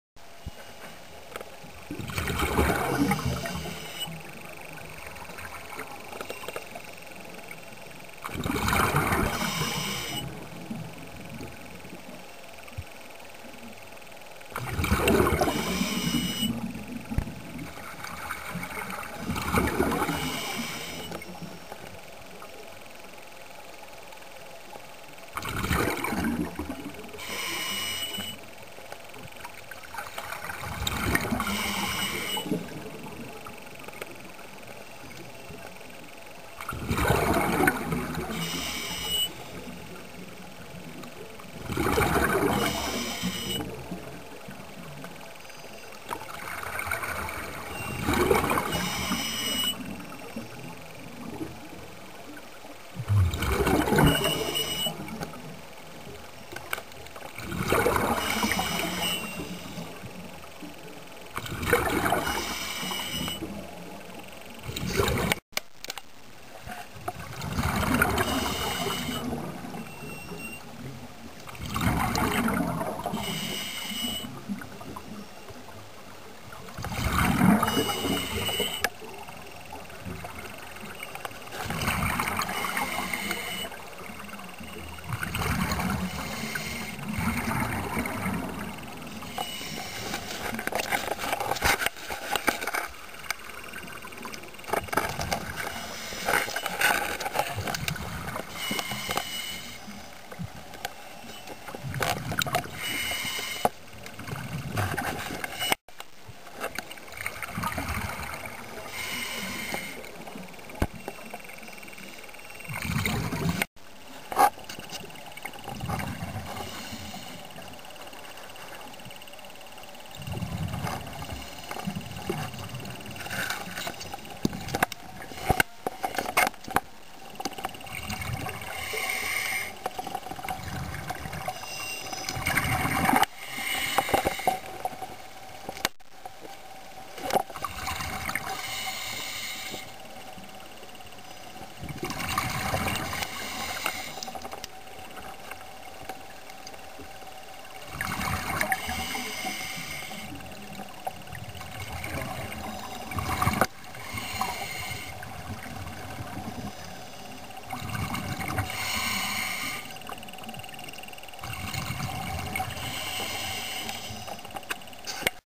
video sous-marine silures en milieu naturel.wmv
filmé par sebastien francois(seb 71 silurus glanis association)